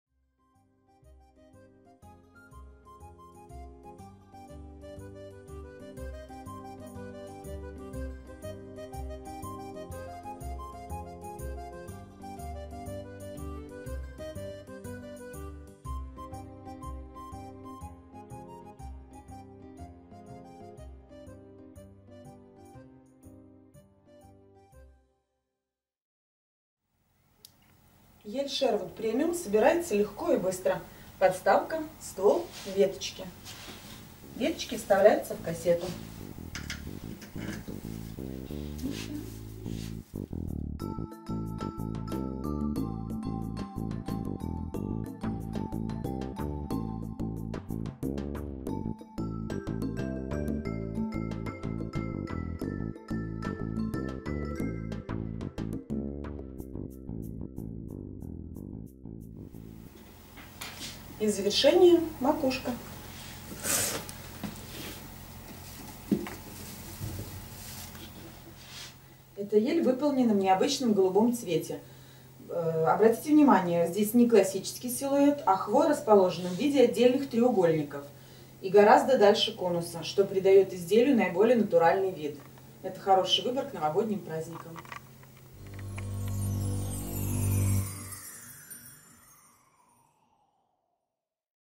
Triumph Tree Ель Шервуд Премиум голубая
Triumph Tree Ель Шервуд Премиум голубая  (Голландия) - краткий обзор от компании ElkaSite.ru